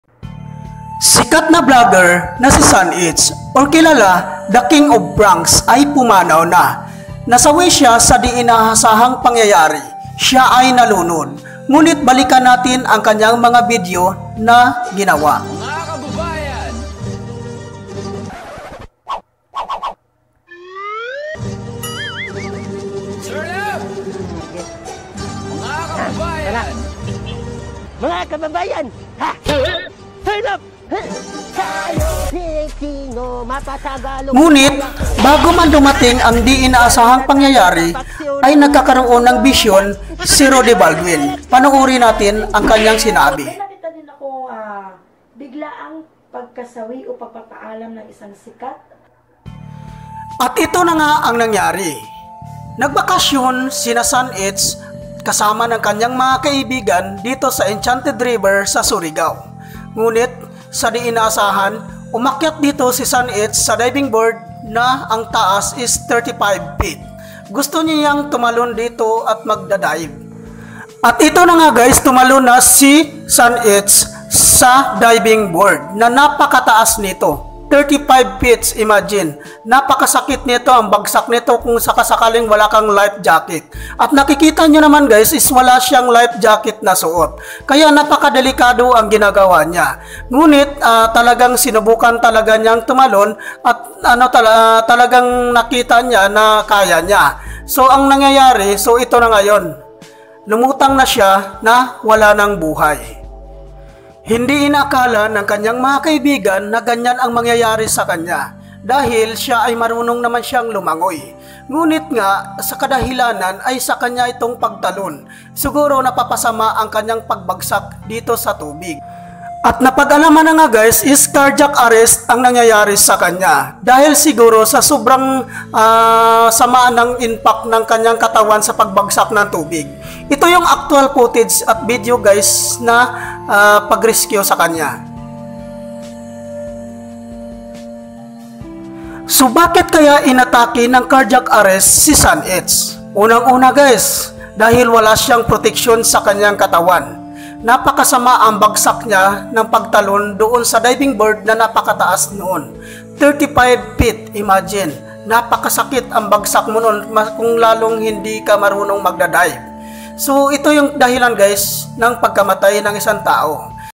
Sikat na vlogger na si San.H nalunod at pumanaw na! | Rudy Baldwin vision nagkatotoo
Sikat na vlogger na si San.H Nalunod at pumanaw na! Rudy baldwin vision nagkatotoo, isang malungkot na balita ito para sa aming mga vlogger na mayroong nasawi sa hindi inaasahang pangyayari,at sa vision naman ni rudy baldwin ay talagang ito na nga ang nakita po niya sa kanyang vision, nakakalungkot man pero kailangan nalang nating tanggapin rest in peace idol San.H#thekingofprankspumanawna